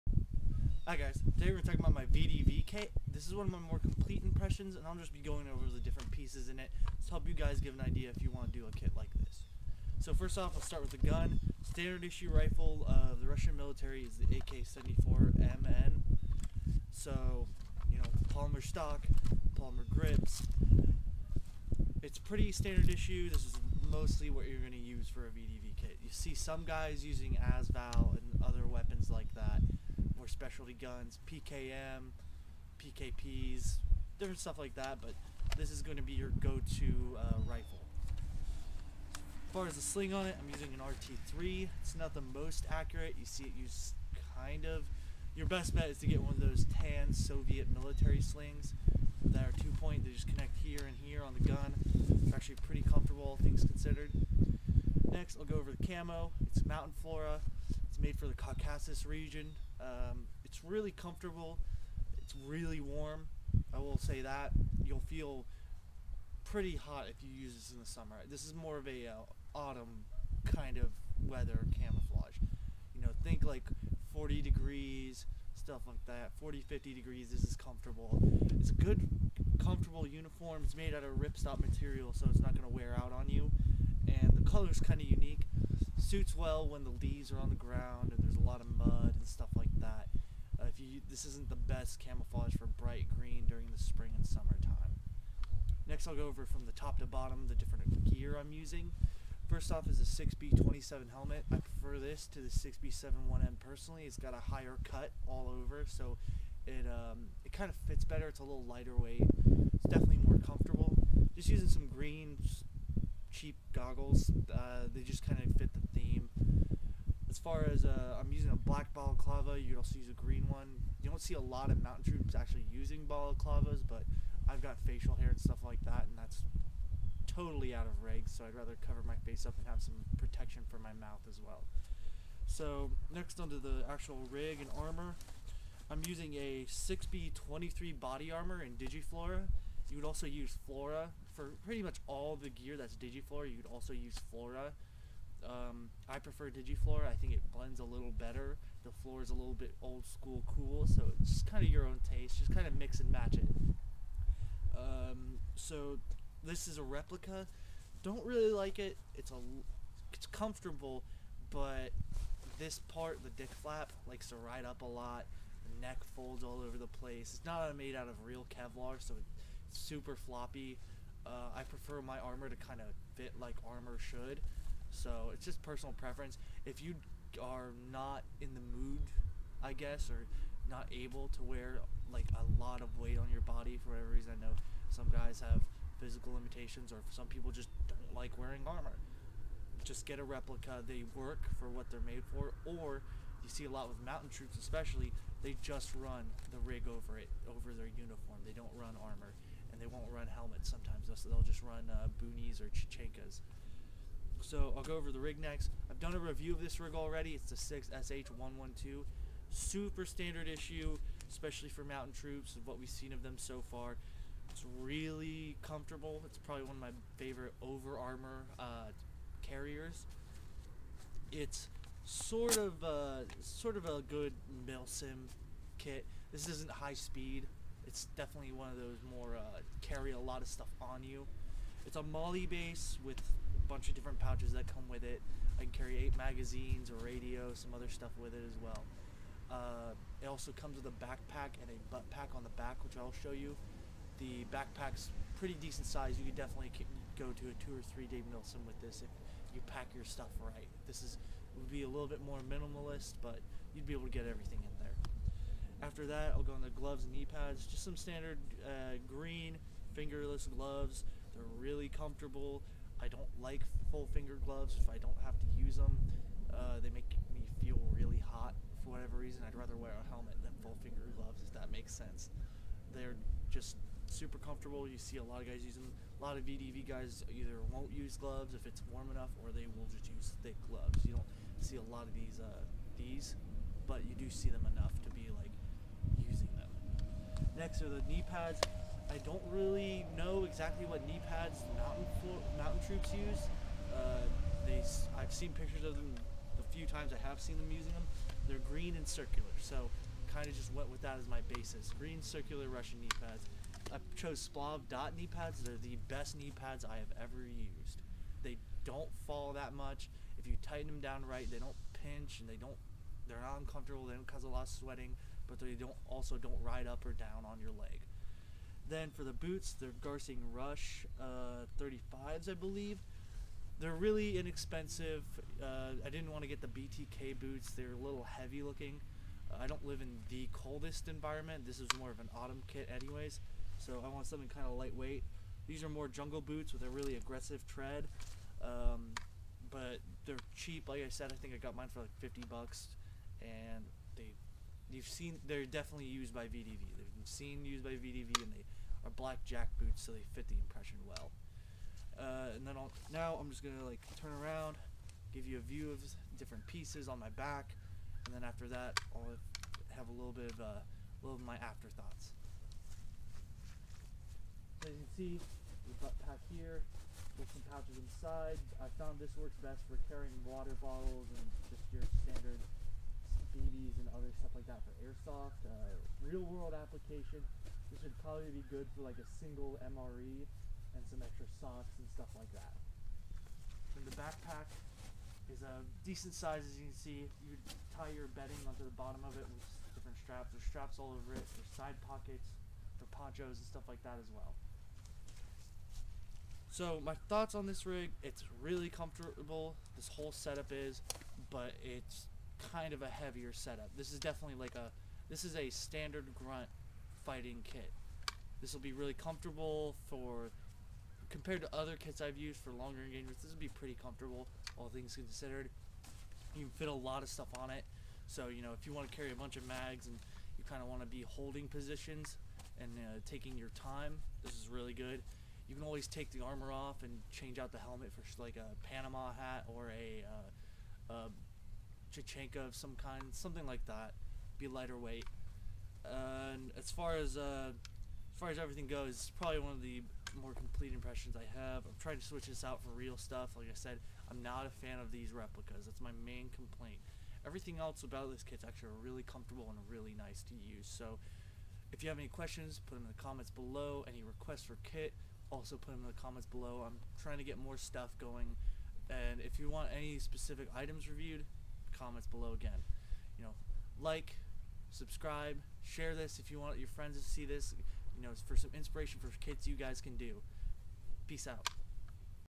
VDV Russian Army Mountain Flora Kit Overview
Got around to making this video before I sold my mountain flora set. Its an amazing and fun kit but as its so seasonal, its hard to enjoy it. Working on some FSB kits right now for future viewing pleasure.